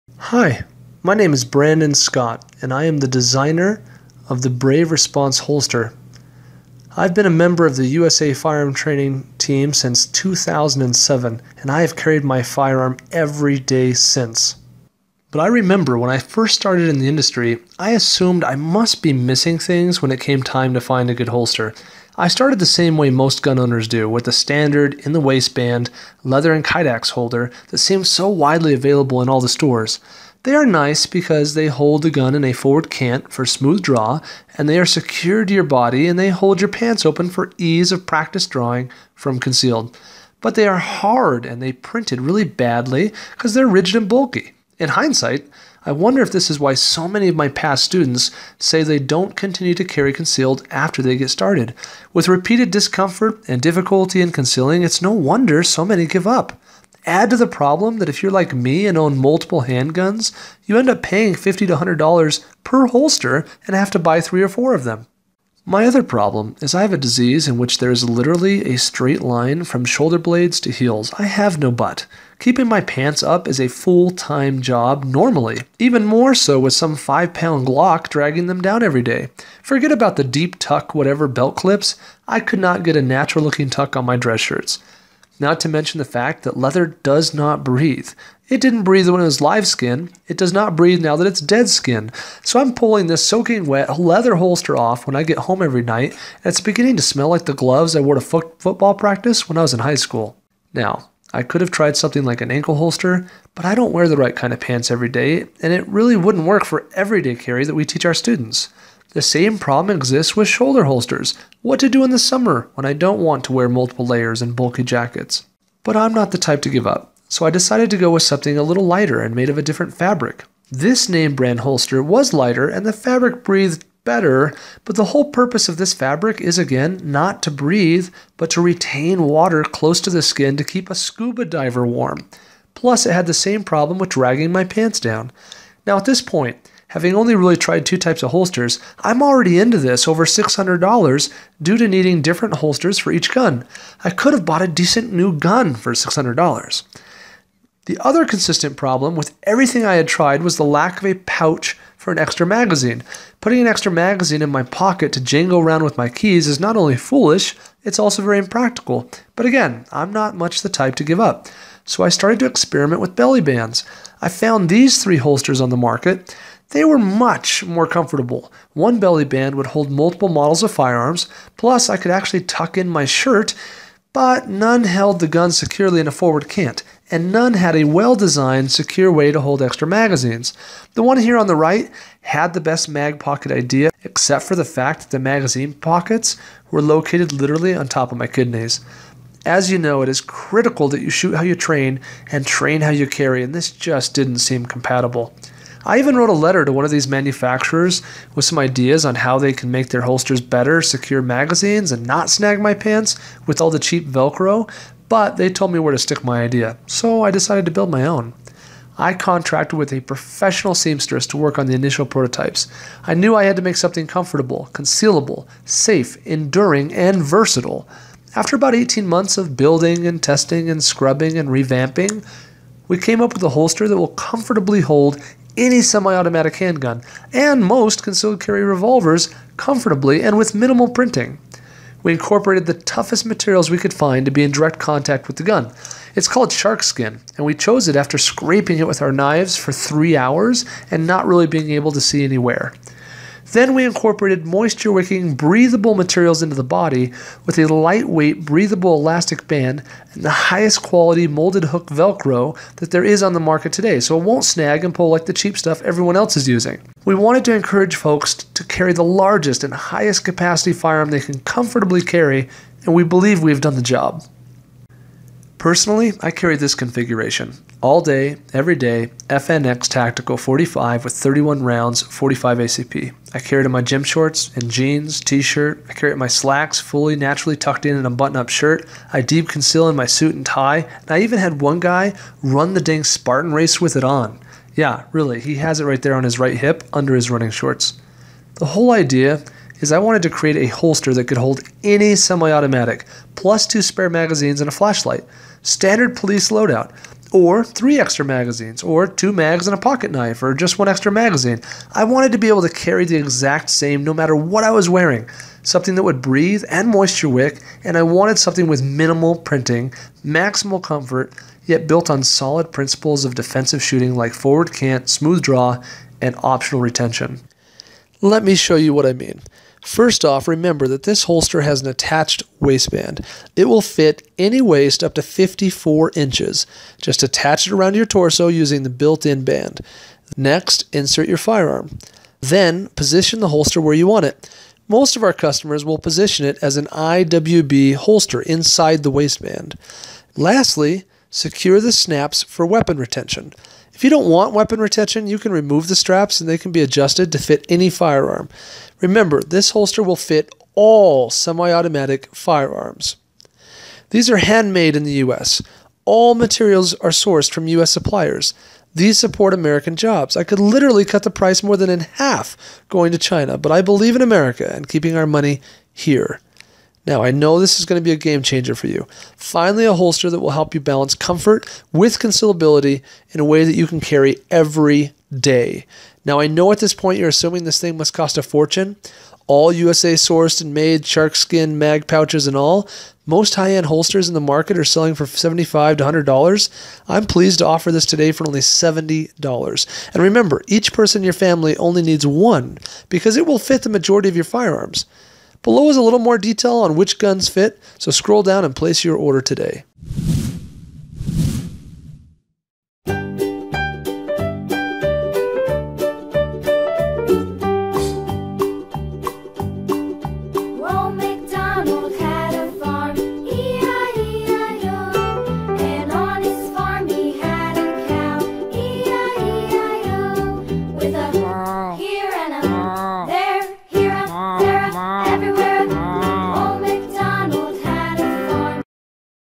Concealed Carry Brave Response Gun Holster
Brave Response Holster My Thoughts. This is day one of this holster review I will give a 3 month update on this holster to see how its holding up and the comfort.